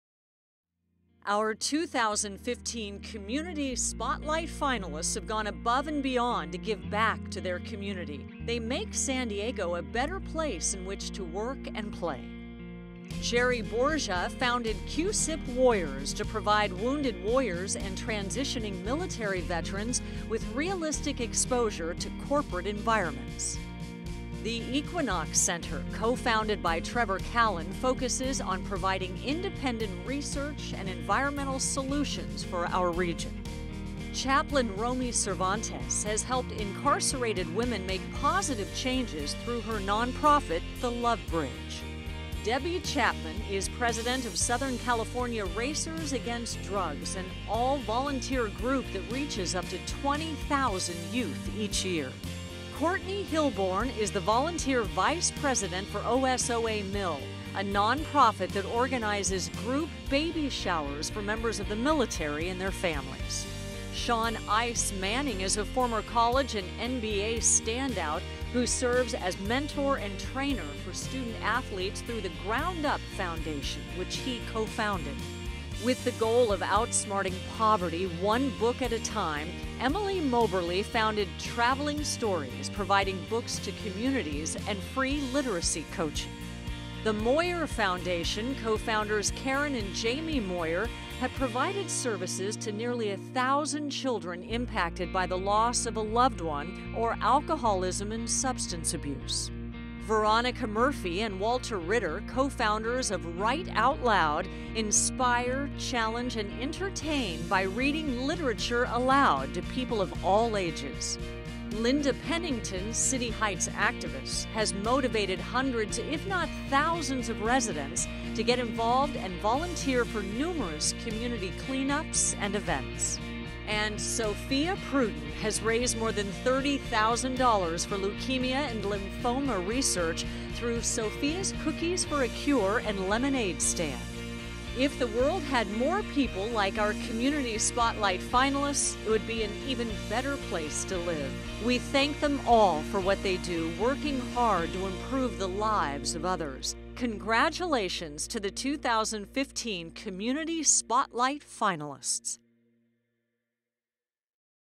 2015 Visionary Awards - Community Spotlight Award Finalists
Find out about the 11 deserving candidates who were finalists for the Community Spotlight Award and whose work was recognized at the 2015 Visionary Awards.